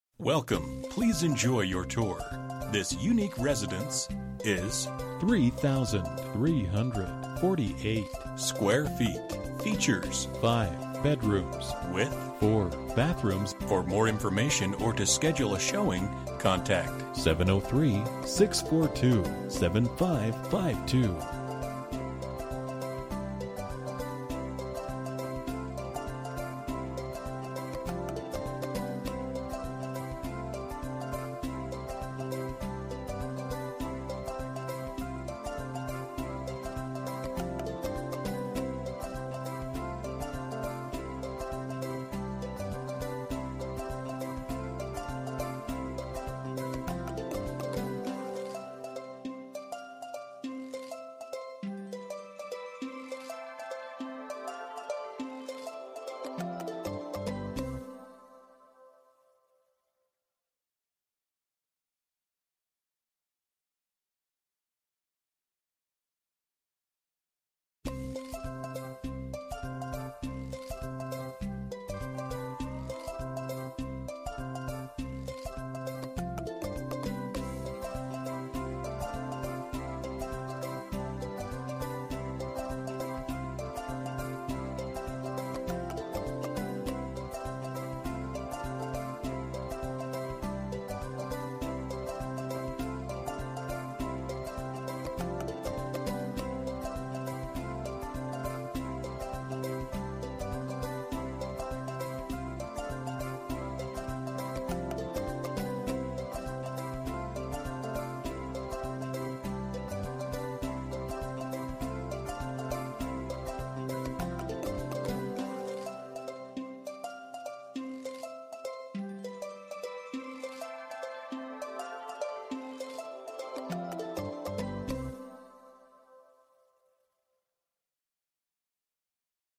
Homes for sale - 3210 GLENWOOD PL, Falls Church, VA 22041
OPEN SUNDAY 6/19, 2-4PM! Over 3300 sqft! Well cared for & updated for you. 1/3 + acre private level yard. Hardwood flrs. Fresh paint. Sun drenched, 5th BR/ in-law suite.  Updated kit. w/ granite & SS. 1st fl. laundry, 2 fireplaces!  Extra storage. New 2 tiered deck & AC. Newer roof, windows & furnace. Walk out rec rm. So convenient-Minutes to Arlington, metro, excellent restaurants & shopping!

Bedrooms: 5
Bathrooms: 4 Bathrooms
Square Feet: 3348
Price: $769,990

MLS ID: FX9687049